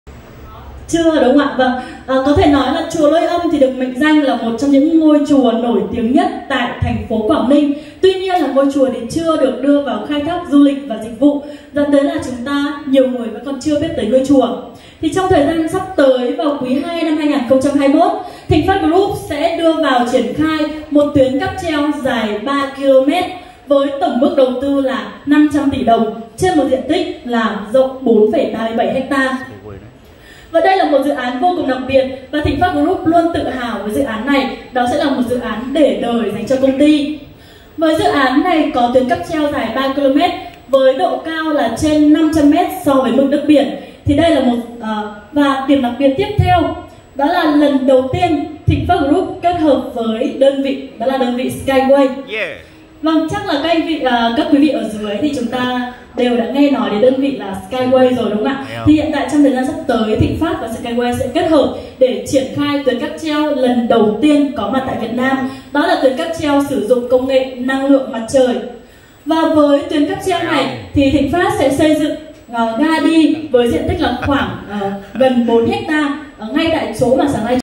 Dự án skyway đường dây trên không tại chùa Lôi Âm
nếu xây dựng điểm này sẽ là ga đi của du lịch sinh thái tâm linh chùa Lôi Âm